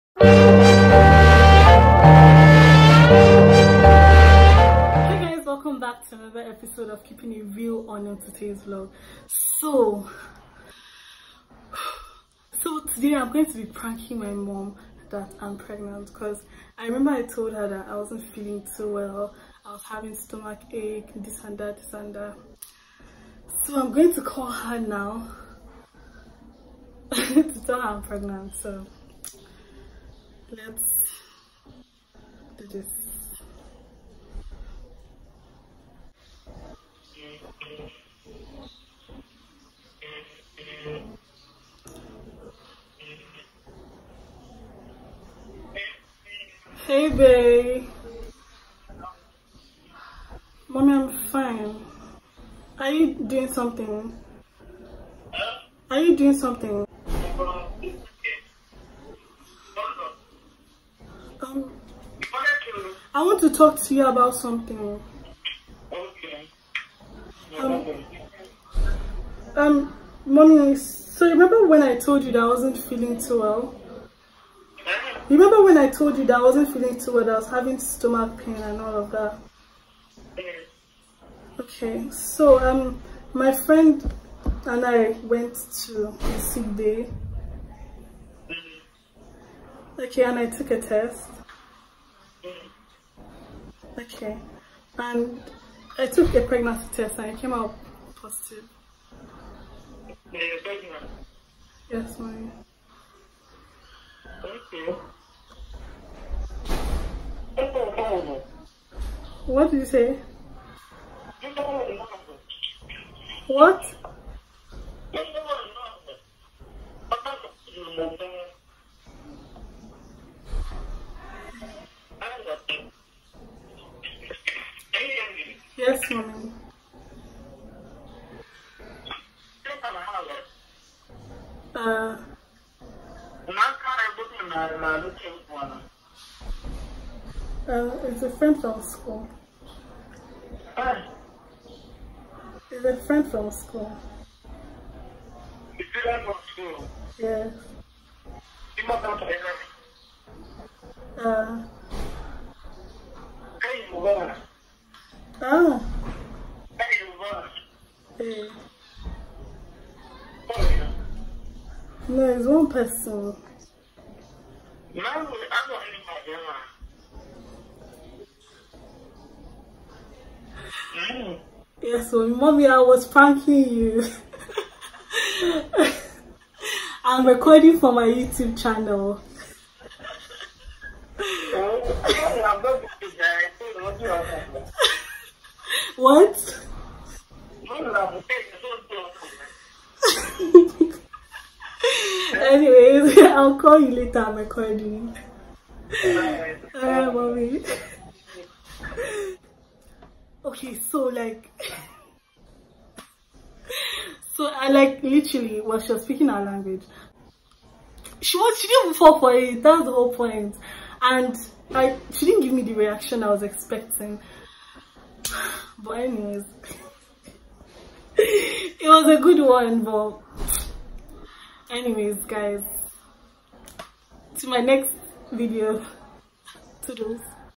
PREGNANCY PRANK ON MY AFRICAN MOTHER !!! *Gone wrong?🙀
This really made my heart skip so fast 😂

Instagram and twitter -  msutaay